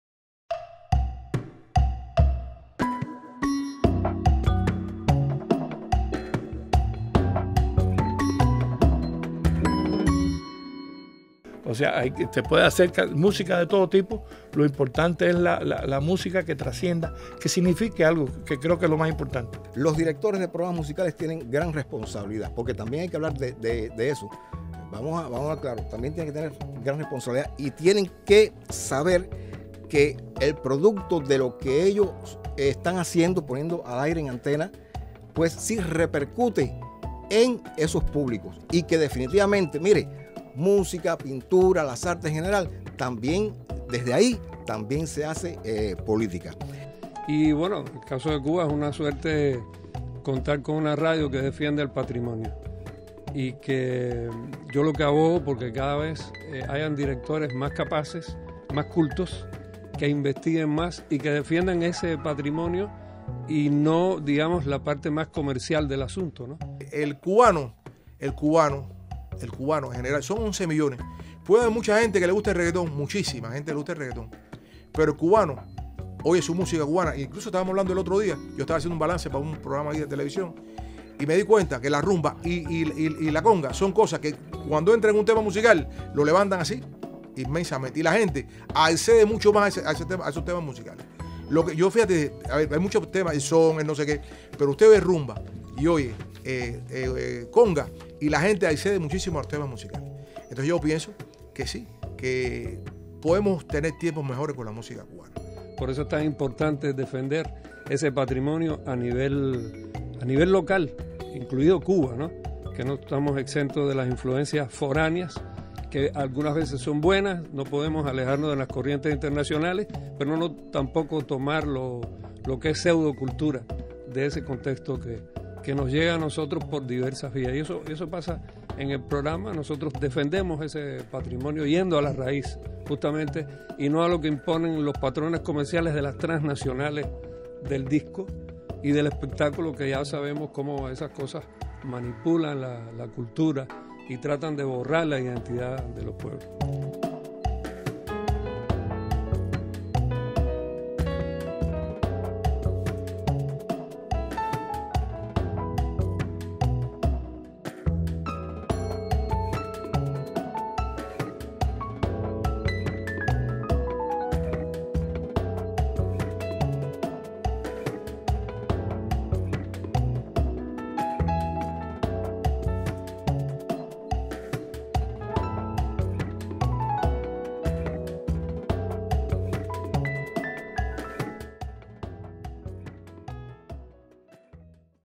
Muy pronto en nuestro canal Moviendo los caracoles... Debate sobre la politica musical de la radio.
Espacio para el debate y el pensamiento crítico de la Unión de Escritores y Artistas de Cuba.